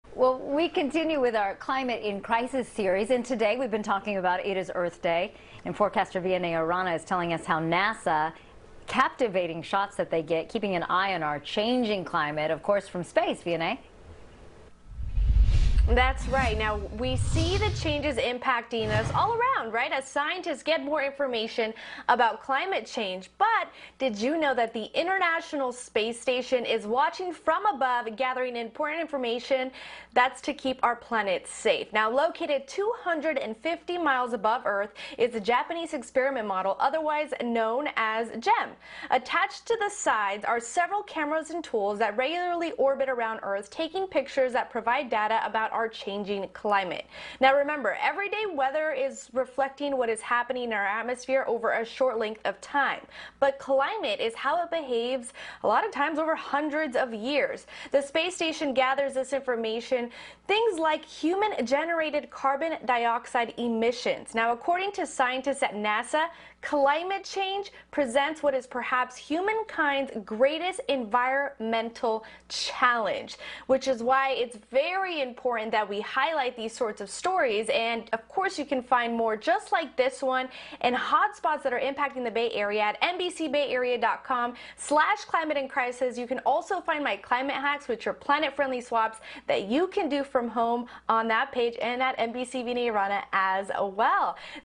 NASA Scientists Track Climate Change From International Space Station
The effects of climate change over time are being tracked by NASA scientists through images taken from the International Space Station. Vianey Arana reports.